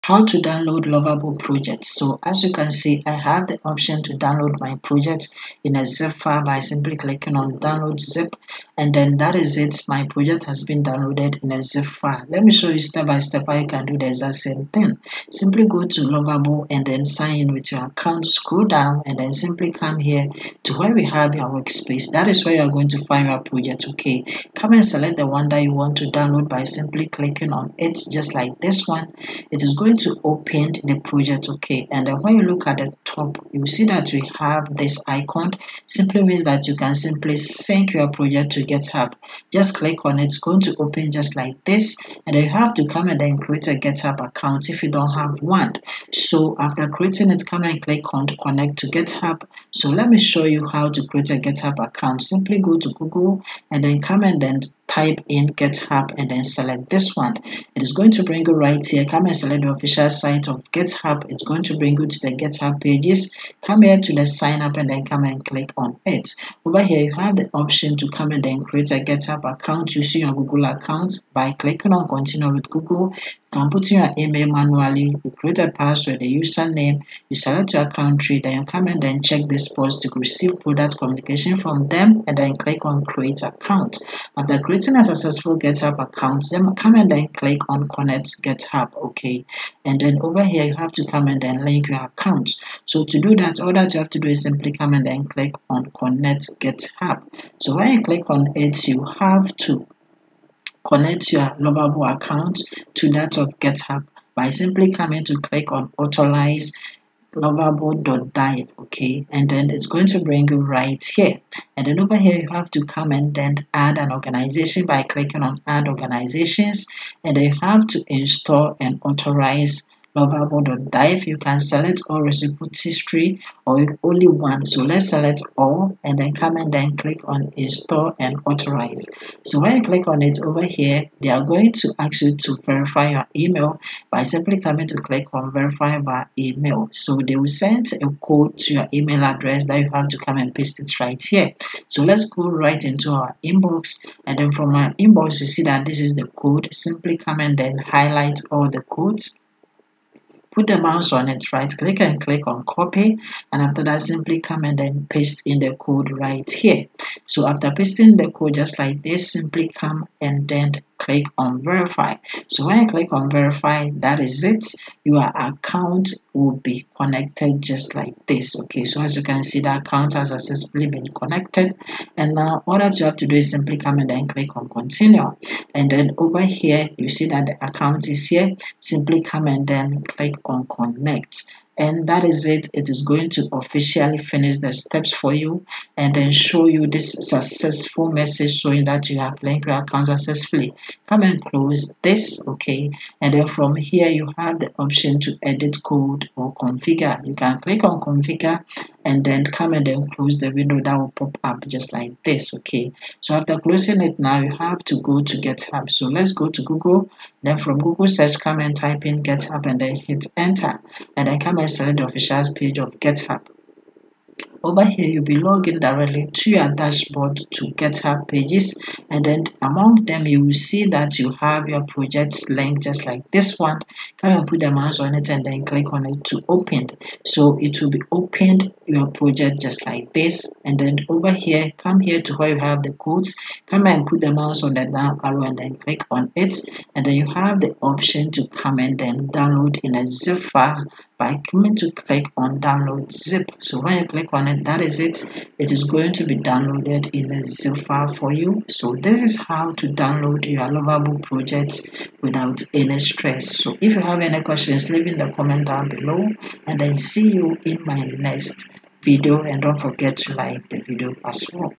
How To Download Lovable Project (No Stress Guide!)
Struggling to download your Lovable AI projects after building them? This "no stress guide" is specifically designed for beginners, showing you exactly "How To Download Lovable Project" built with Lovable AI, fast and easy.

Learn the simple, step-by-step process of connecting your GitHub account to Lovable Dev, allowing you to seamlessly download your complete app as a convenient zip file directly from GitHub, ensuring a completely stress-free experience.